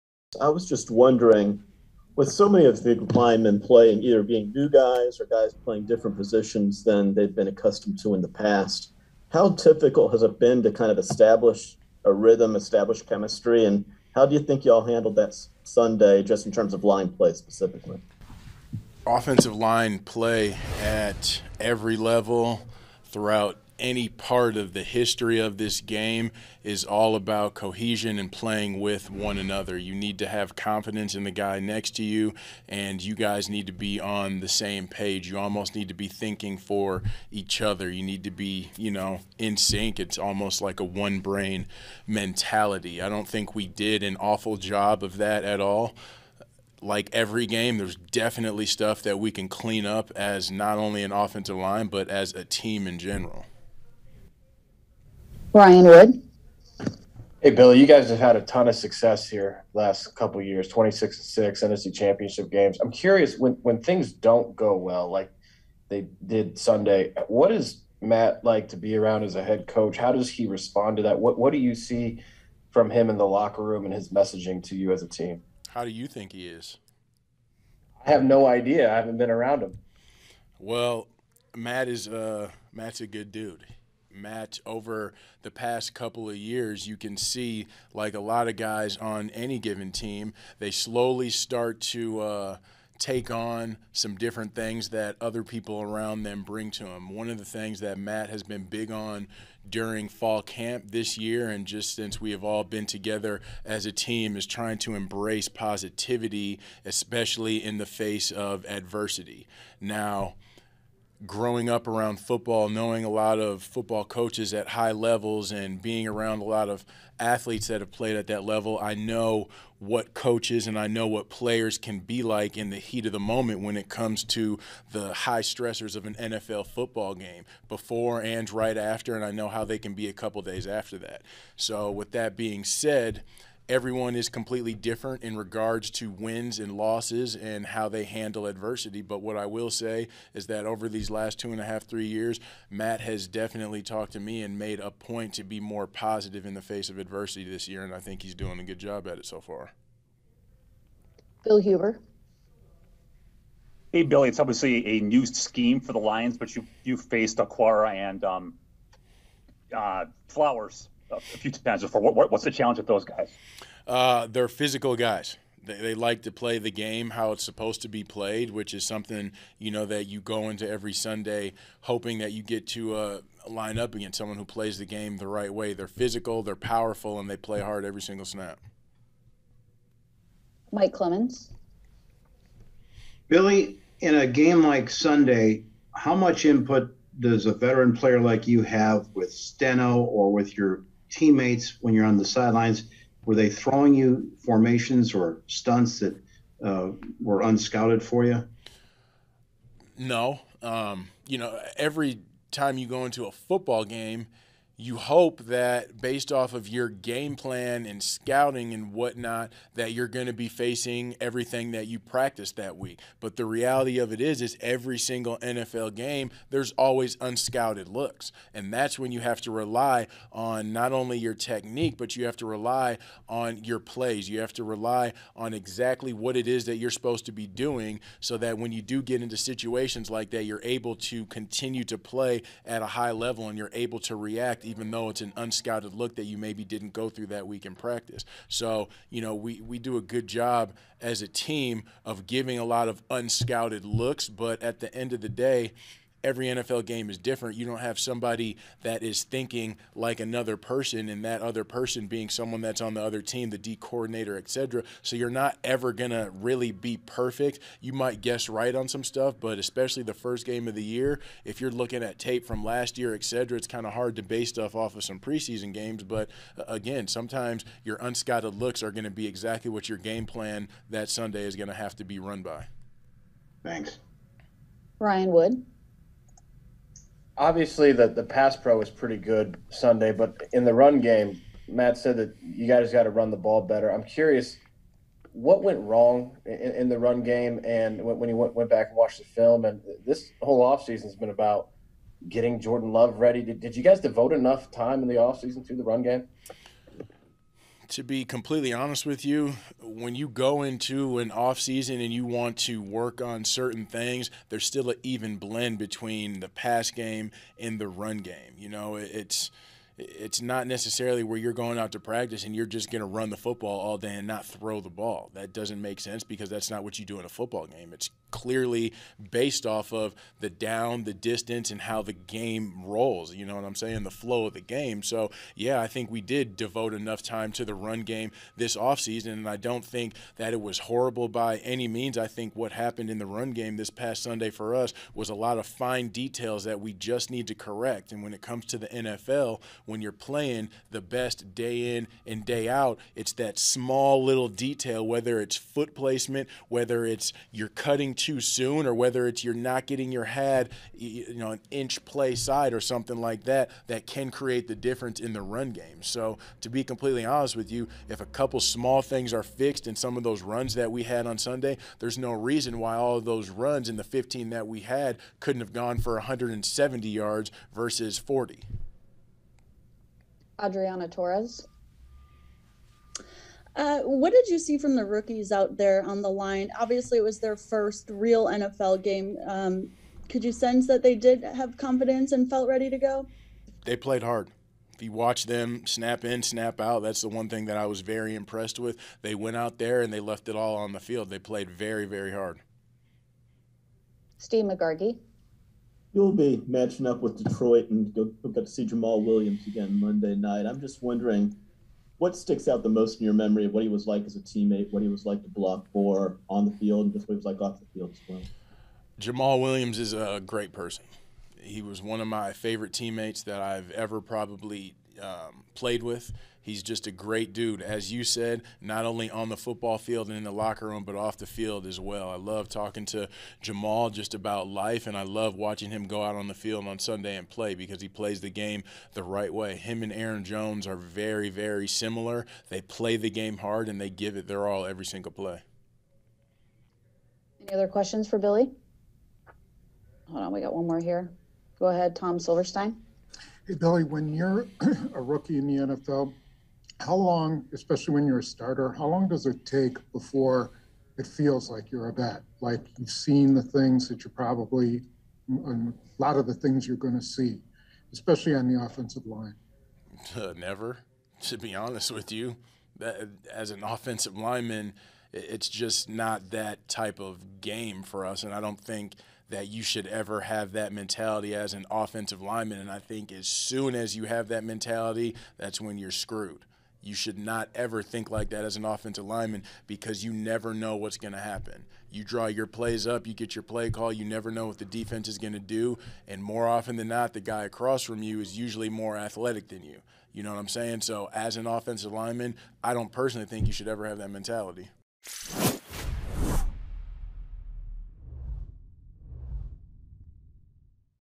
Turner: ‘Fine Details We Need To Correct’ In The Run Game
G/T Billy Turner spoke to the media on Tuesday as the Packers look to turn the page towards Week 2.